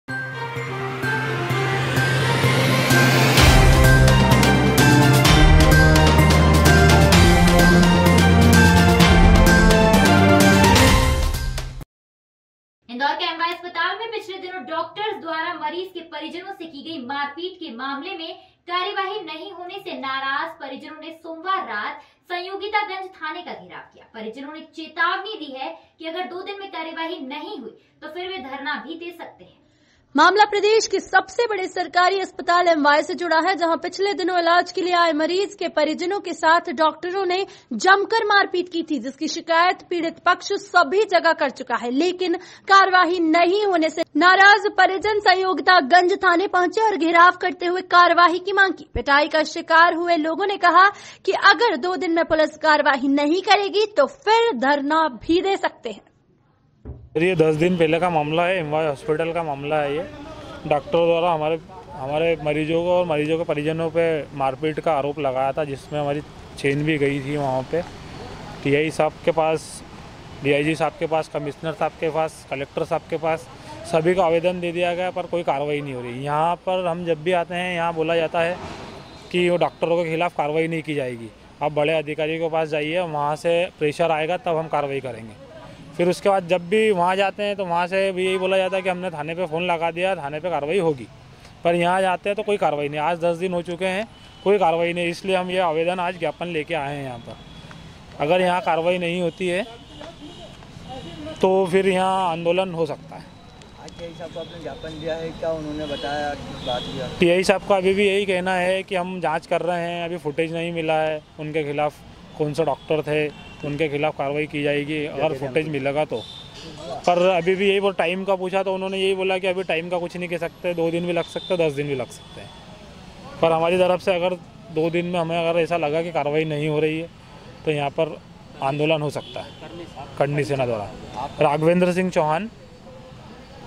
इंदौर - एमवाय मारपीट मामला
इंदौर के एमवाय अस्पताल में पिछले दिनों डॉक्टरों द्वारा मरीज के परिजनों से की गई मारपीट के मामले में कार्यवाई नही होने से नाराज परिजनो ने सोमवार रात संयोगितगंज थाने का घेराव किया । परिजनों ने चेतावनी दी है कि अगर दो दिन में कार्यवाई नही हुई तो फिर वे धरना भी दे सकते है ।

मामला प्रदेश के सबसे बड़े सरकारी अस्पताल एमवाय से जुड़ा है  । जहाँ पिछले दिनों इलाज के लिये आए मरीज के परिजनों के साथ डॉक्टरों ने जमकर मारपीट की थी । जिसकी शिकायत पीड़ित पक्ष सभी जगह कर चुका है । लेकिन कार्यवाई नही होने से नाराज परिजन संयोगितगंज थाने पहुँचे ओर घेराव करते हुए कार्यवाई की मांग की।

पिटाई का शिकार हुए लोग ने कहा है कि अगर दो दिन में पुलिस कार्यवाई नही करेगी तो वे फिर धरना भी दे सकते है ।